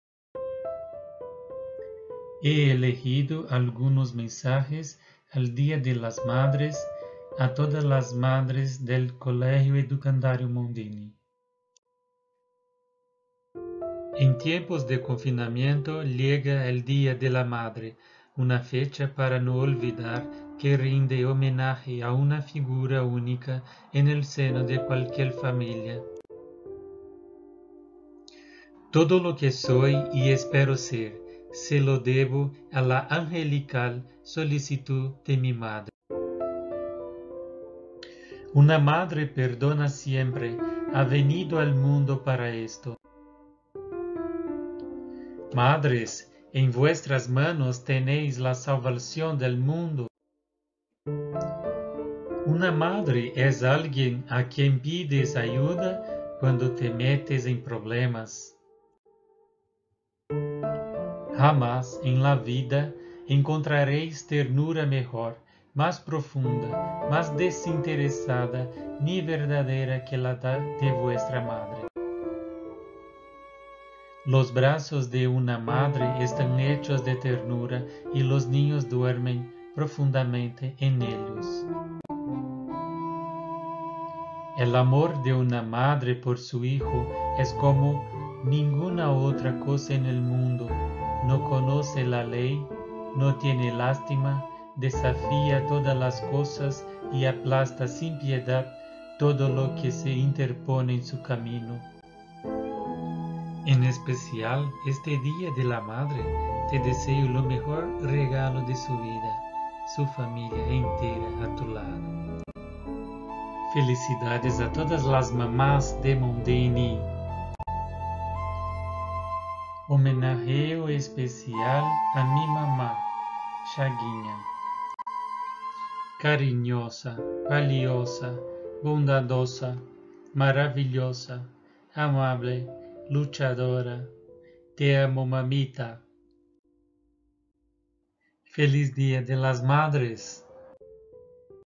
Mensaje Día de las madres 2020
El video es un homenaje especial a mi mamá y a todas las mamás de Colégio Edukandarium Mondini en el año de 2020.